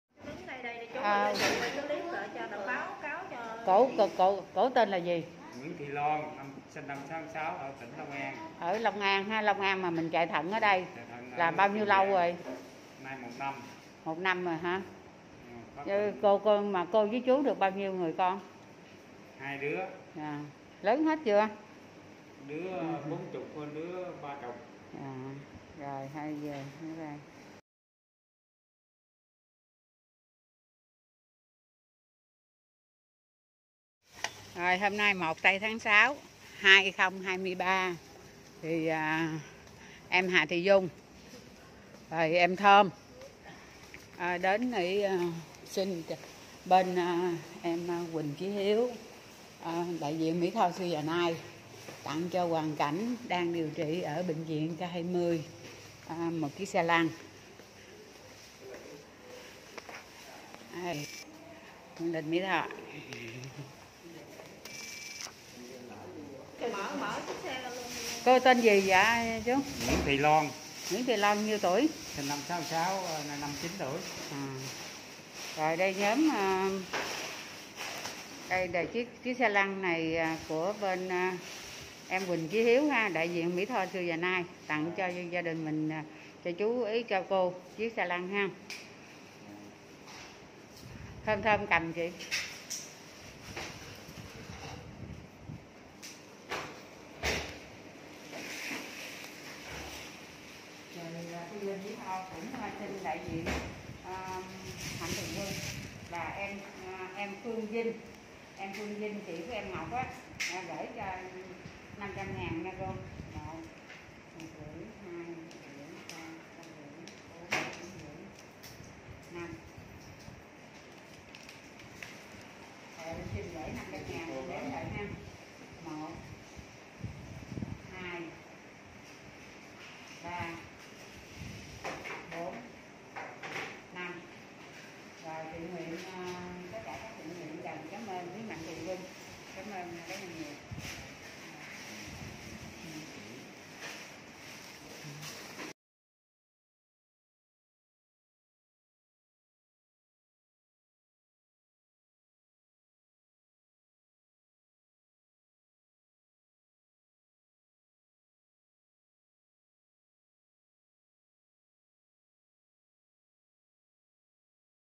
1.6.2023 chị Loan suy Thận BV k120
Hc chị Loan suy Thận. Quý MTQ Em Phương Dinh Trao Yêu Thương 500.000₫. Quý MTQ TQ Em Huỳnh Chí Hiếu trao 1 xe Lăn. Tại BV K120